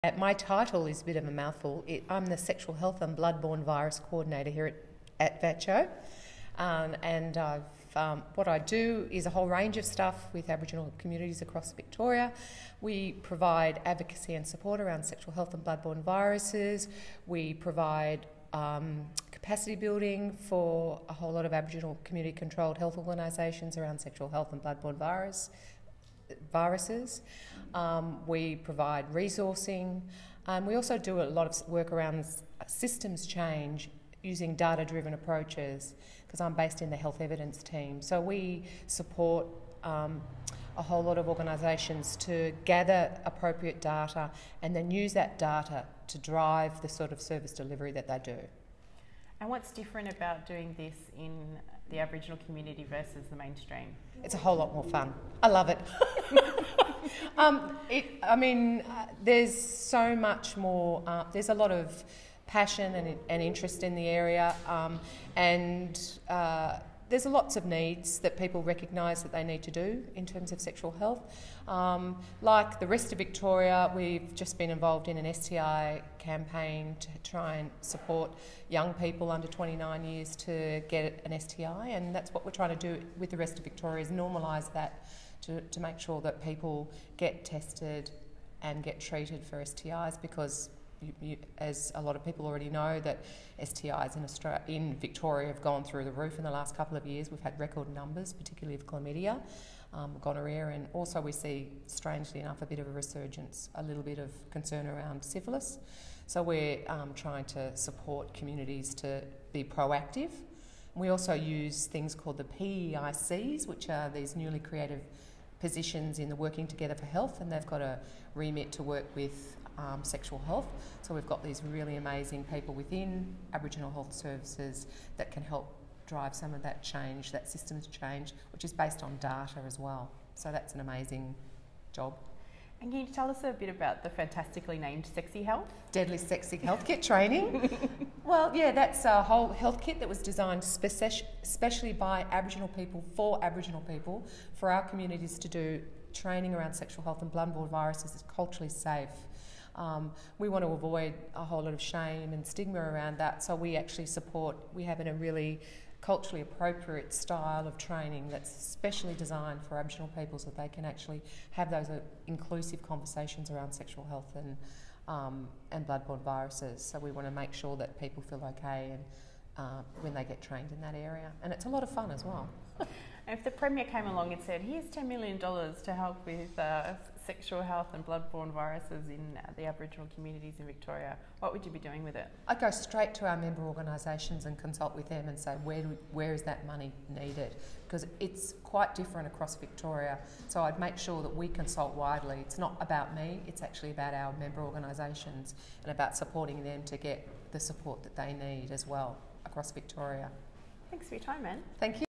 Talking sexual health at VicVotes
Talking about the sexual health program at the Victorian Aboriginal Community Controlled Organisation (VACCHO), including efforts to address rising notifications in Victoria of chlamydia, gonorrhoea and infectious syphilis which reached record highs in 2017. 49 views on Periscope as of 22 November 2018.